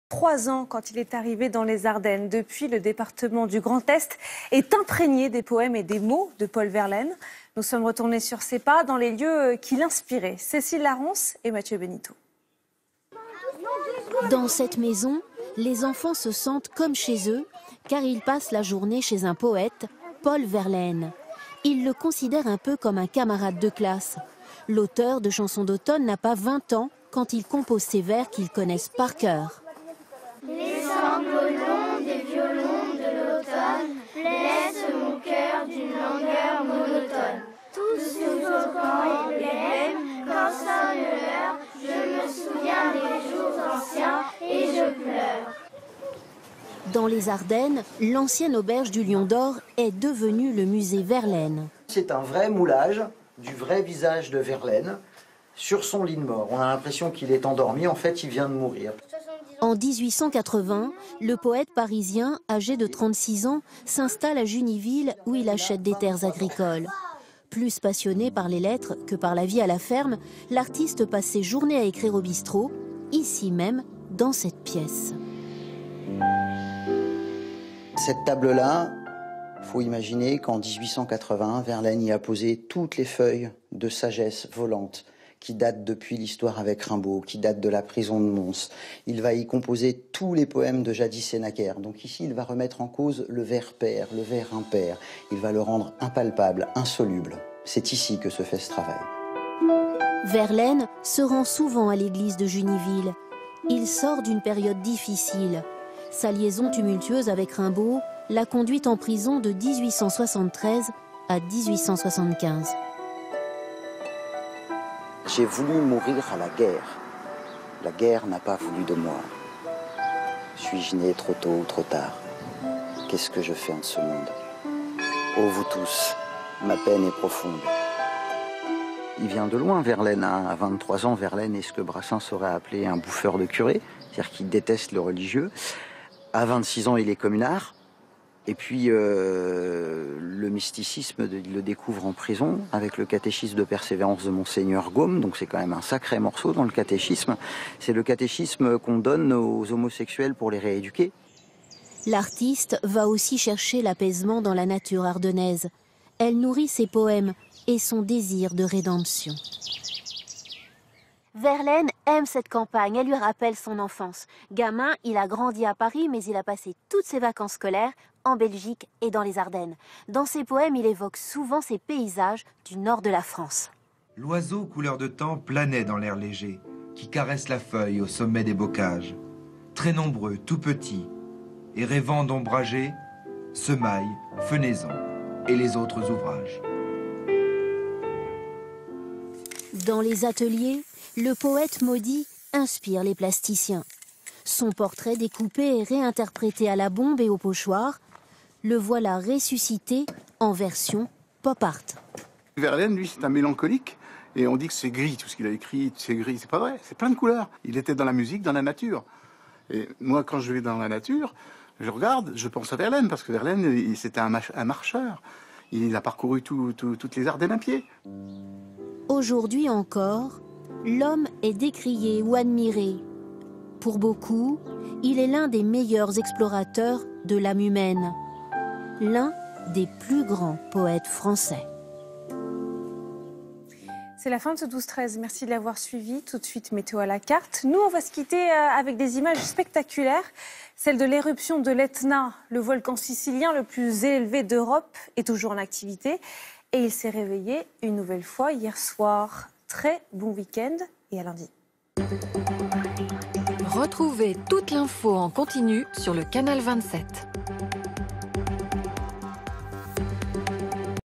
Paul Verlaine : les Ardennes, territoire d'inspiration pour le poète
Reportage Fenêtre sur les Ardennes de Verlaine.
JT 12/13 National du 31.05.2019
A découvrir :
Le Musée Verlaine de Juniville avec Marc GAILLOT
Les portraits Verlaine de l'artiste ardennais Hubert PAUGET : sculptures en inox, bombages